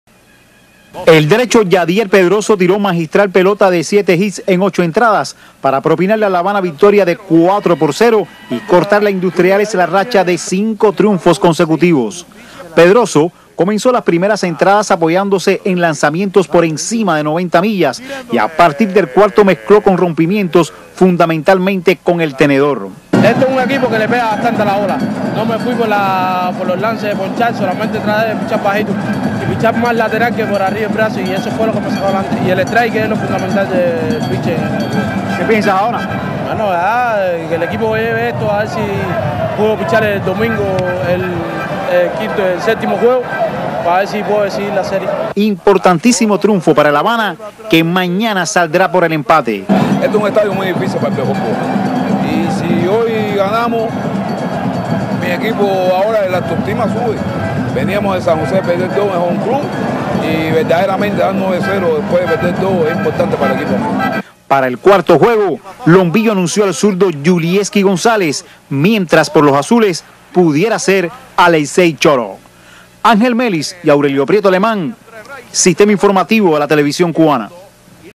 La Habana derrota 4-0 a Industriales durante el tercer partido de la final occidental
La Habana,-. Apoyados en un brillante trabajo del derecho Yadier Pedroso, los Vaqueros del Habana derrotaron hoy 4-0 a los Leones de industriales y obtuvieron así su primera victoria en la final Occidental del béisbol cubano.  Pedroso, tercer abridor de la rotación del Habana, trabajó ocho capítulos a un ritmo demoledor, combinando con excelencia su vasto repertorio de lanzamientos. Durante su actuación de esta fecha en un abarrotado Latinoamericano, el internacional habanista propinó tres ponches, regaló par de boletos y permitió siete hits.